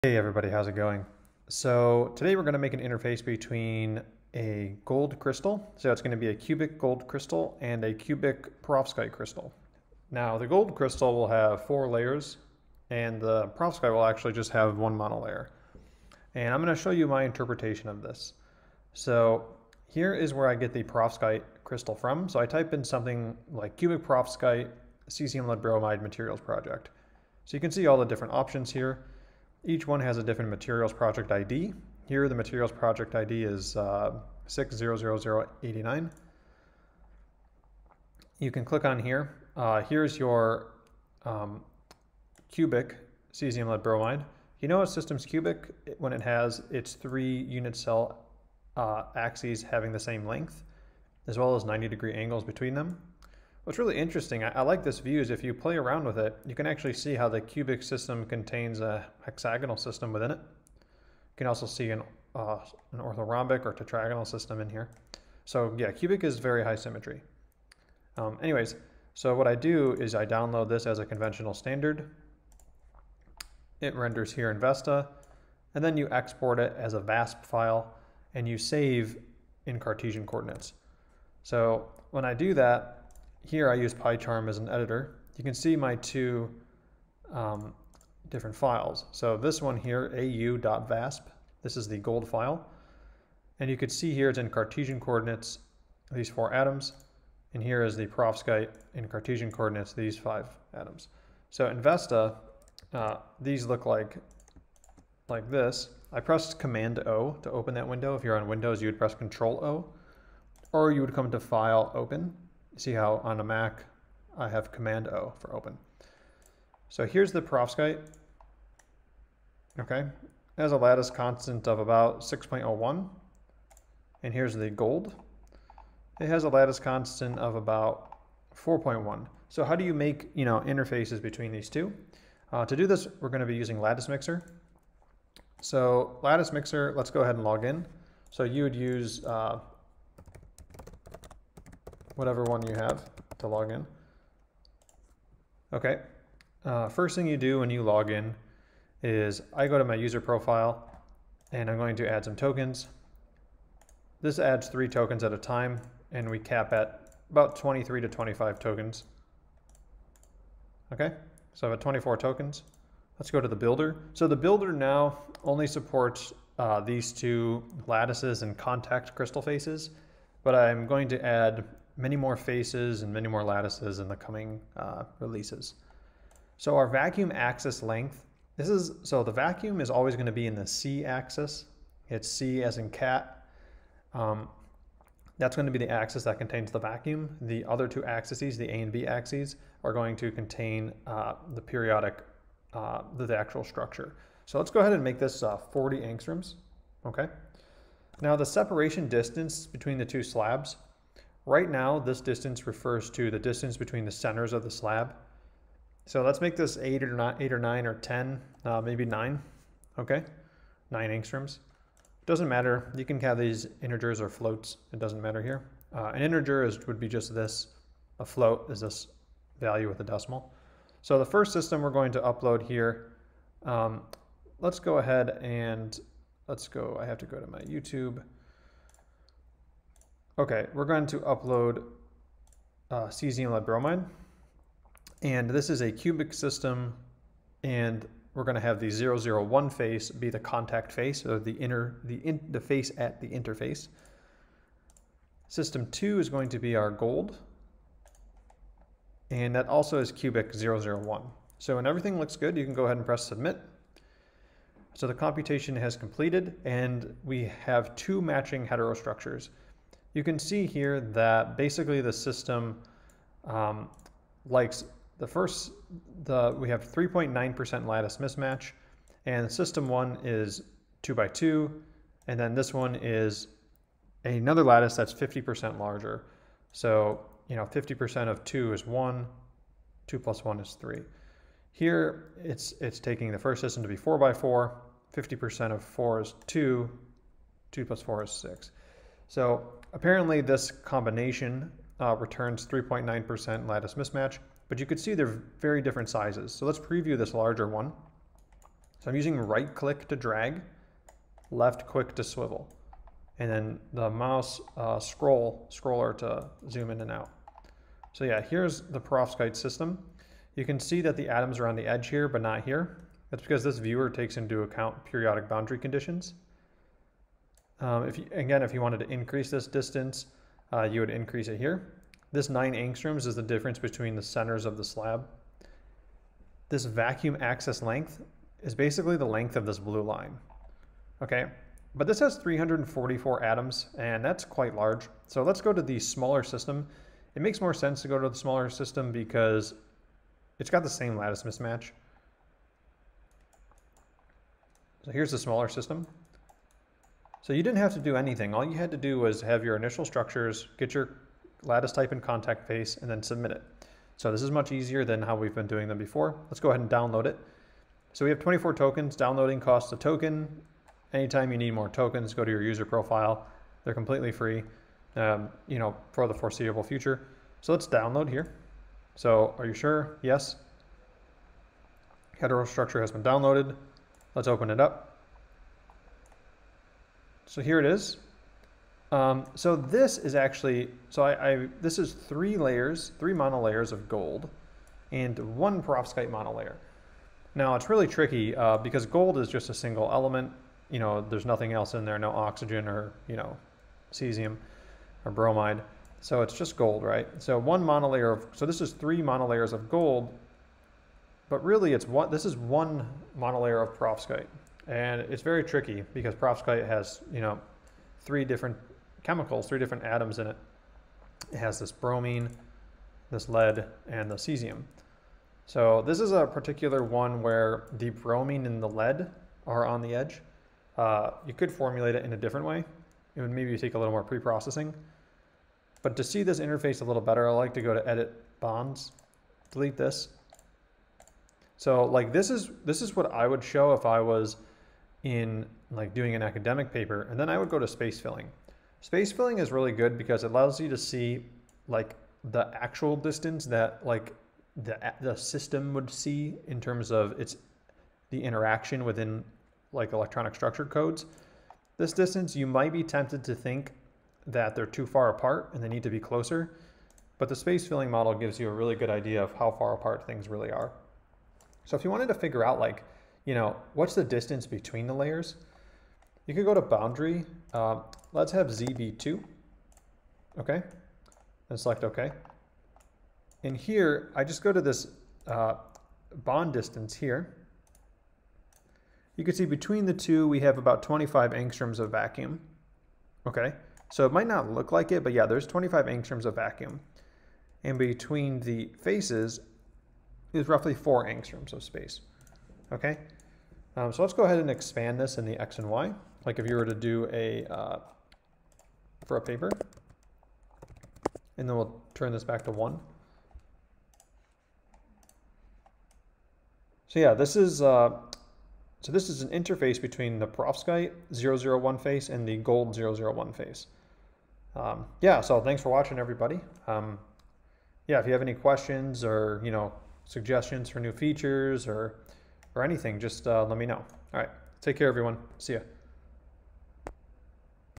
Build Interface: cubic CsPbBr3 (001) / cubic Au (001) with LatticeMixer and VESTA
In this video, we use LatticeMixer and VESTA to build and analyze an interface between cubic CsPbBr3 (001) and cubic Au (001).

Please note that the perovskite crystal surface here may is not stoichiometrically balanced nor charge neutral (assuming native oxidation numbers). One likely needs to add atoms/molecules to passivate dangling bonds on the surface of the perovskite crystal and bring charge neutrality to the perovskite layer. Despite this, there are works that have used a similar approach to perovskite slabs, and making such models is an active area of research.